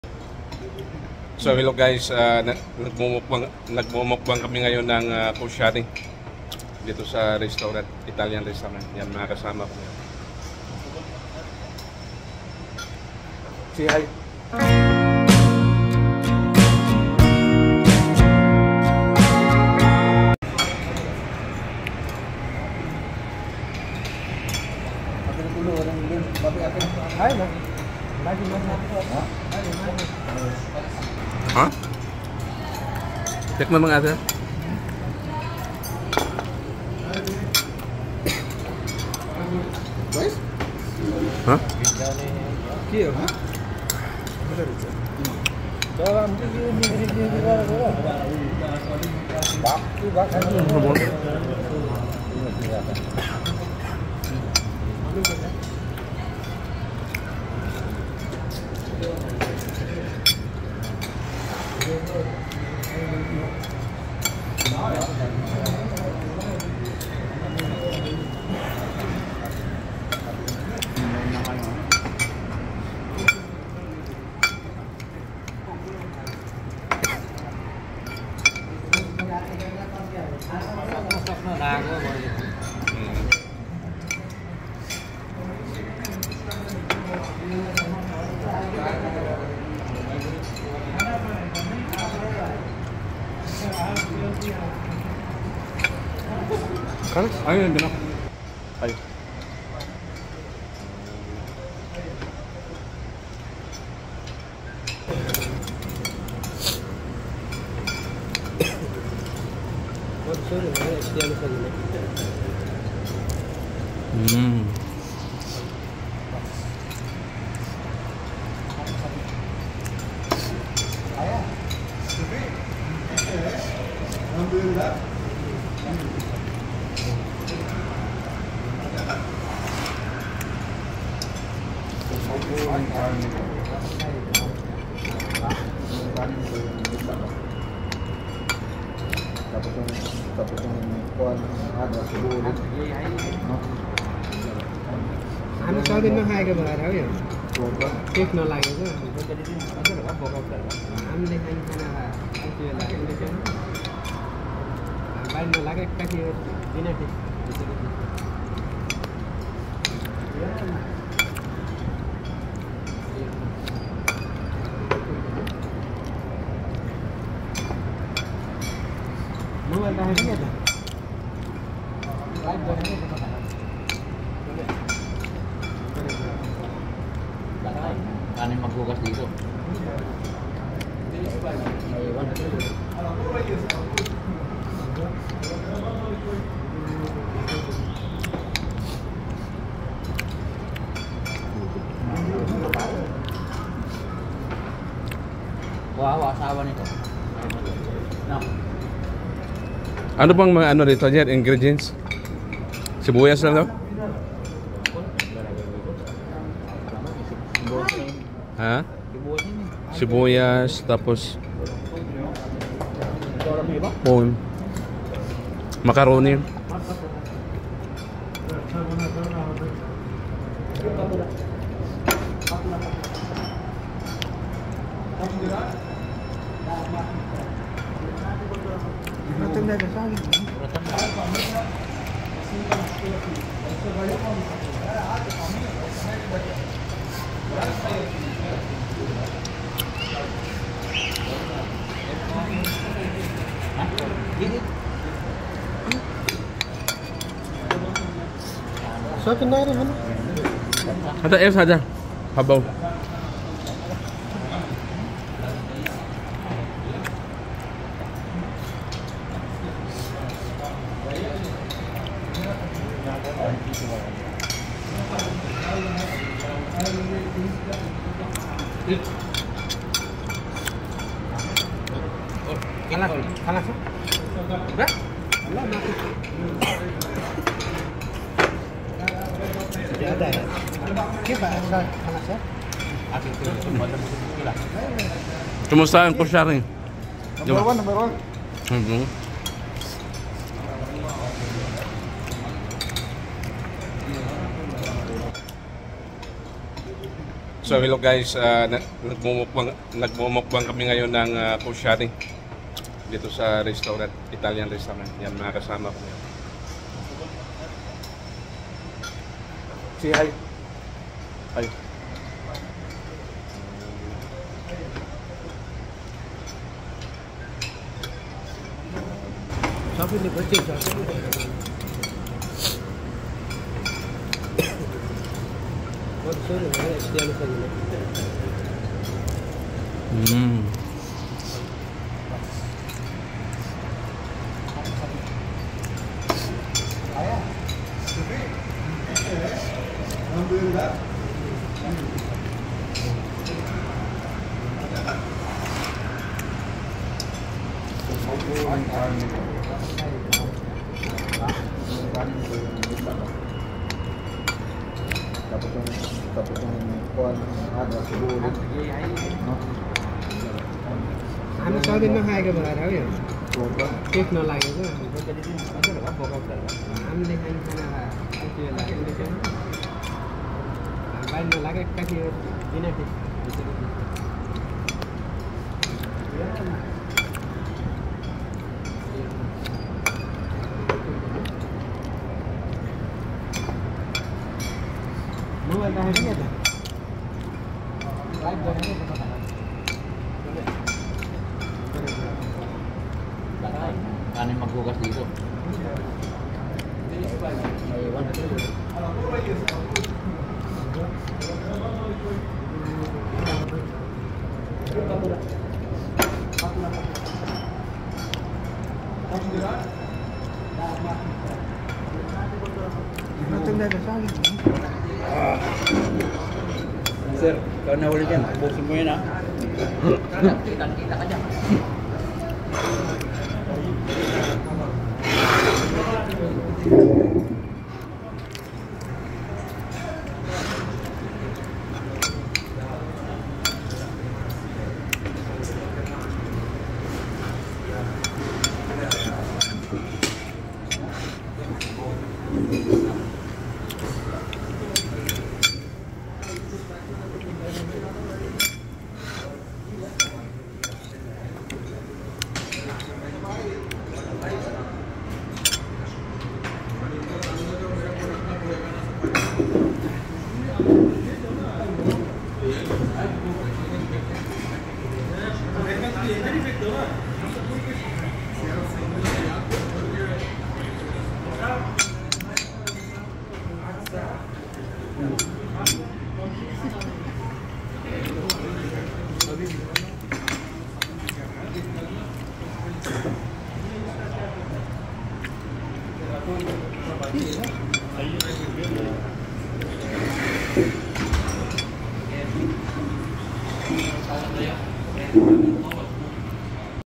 Kushari Mukbang the National Dish of Egypt Masarap kapag sabay sabay kumain Ofw Life in Saudi Arabia
Koshary food